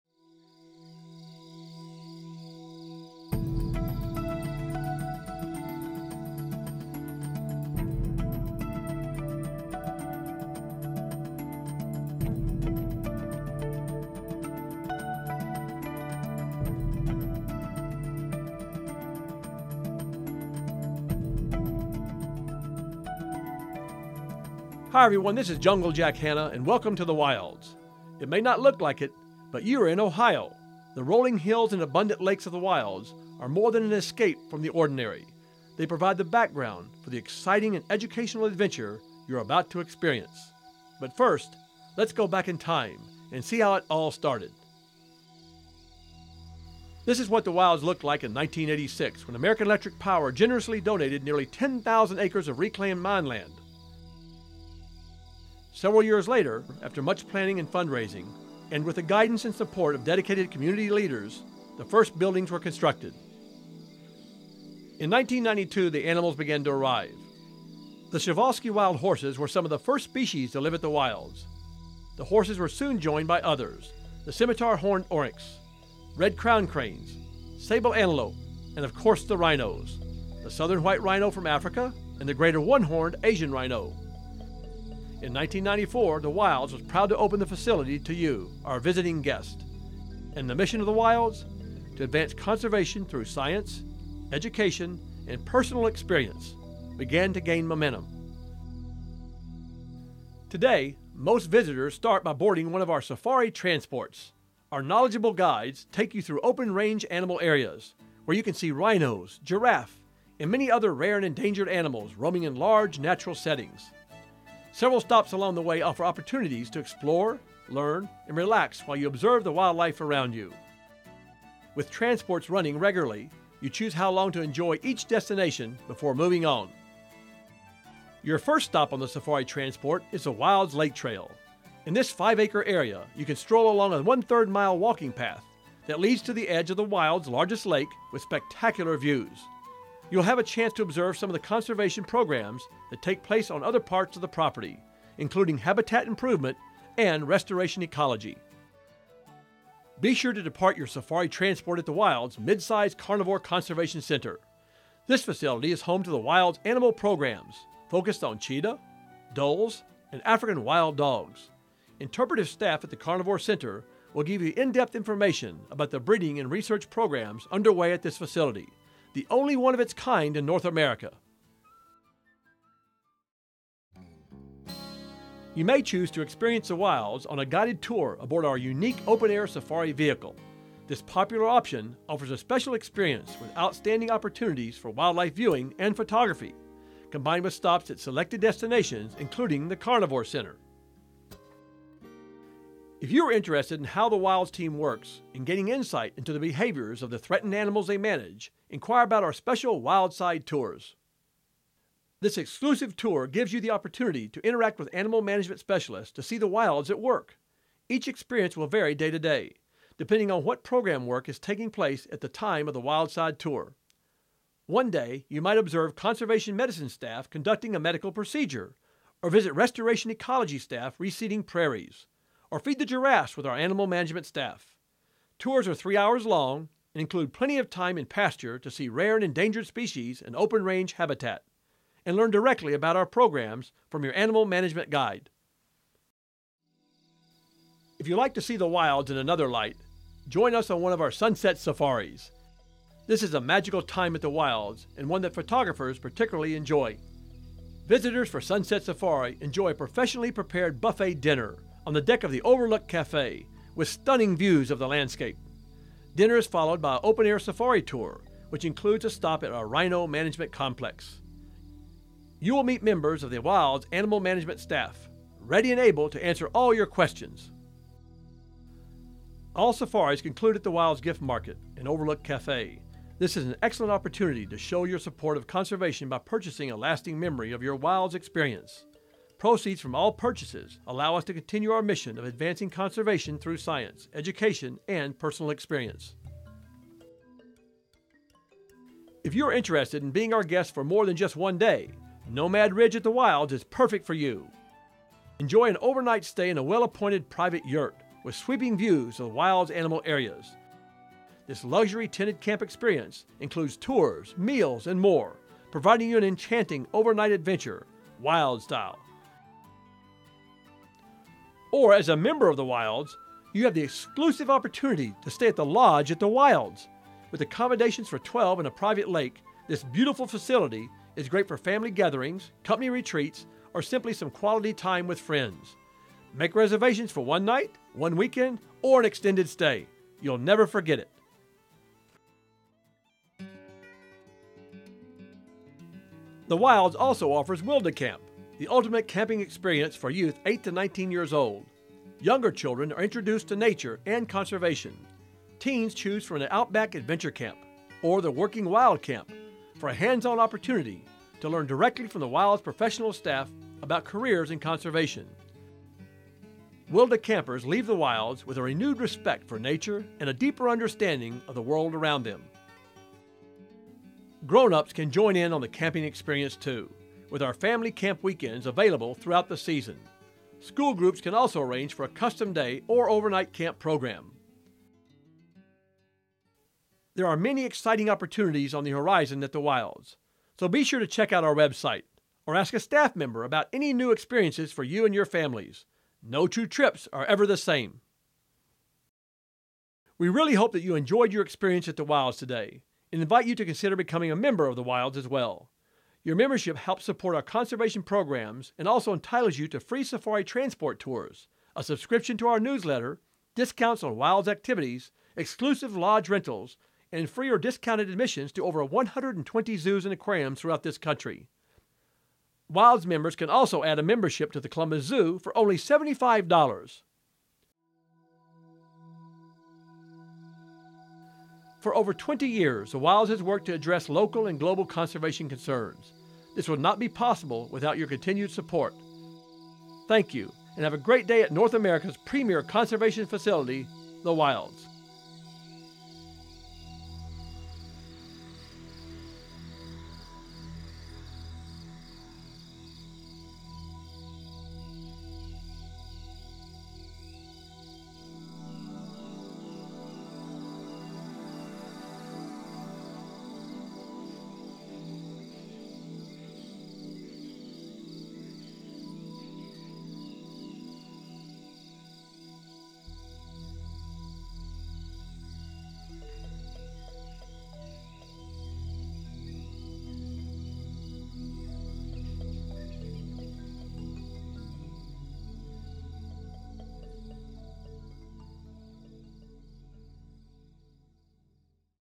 Welcome to the Wilds
The Wilds is one of the largest and most innovative wildlife conservation centers in the world. Located on nearly 10,000 acres in southeast Ohio, it is home to rare and endangered species from around the world living in natural, open-range habitat, as well as home to hundreds of indigenous species. In addition to innovative, in-depth science and education programs, the Wilds offers guided experiences to the public on selected days May through October. for more information.